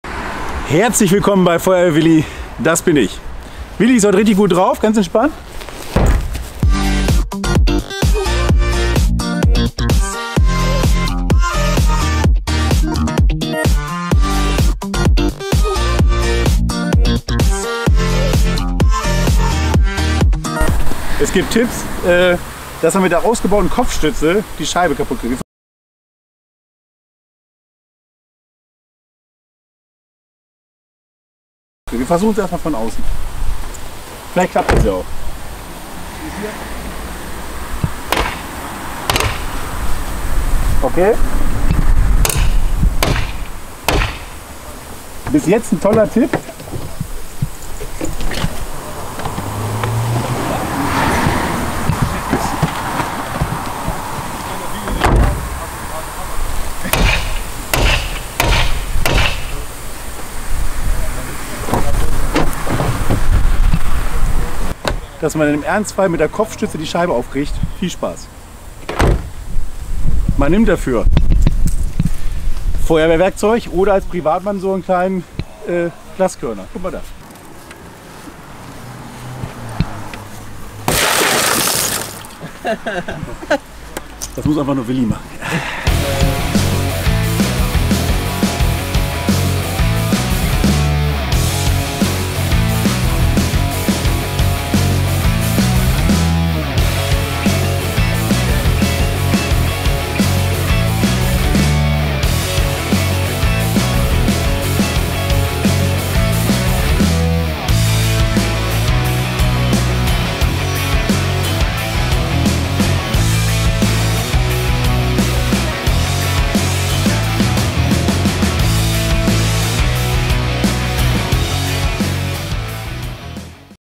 Selbstrettung mit Kopfstütze - ANLEITUNG
FEUERWEHRWILLI hat sich oft geärgert über total dämliche Tipps im Internet.

Normalerweise ignoriert Willi das höflich und respektvoll. Nur hier kann man nicht "Leben retten", sondern "gefährden!".

Diese Falschaussage mit der Kopfstütze kann Willi nicht stehen lassen, aus 2 Gründen:

1. Bei vielen Fahrzeugen, bekommt man die Kopfstütze ohne Werkzeug (z.B. Schraubendreher) nicht aus der Rückenlehne ausgebaut (Beispiel ab VW Golf 4).

2. Sieht man im Video was passiert, wenn man beherzt mit der Kopfstütze auf die Scheibe schlägt - NICHTS!

Bitte wenn dann richtig vorbereiten. Glaskörner (RescueMe z.B.) beriet legen oder am Schlüsselbund befestigen.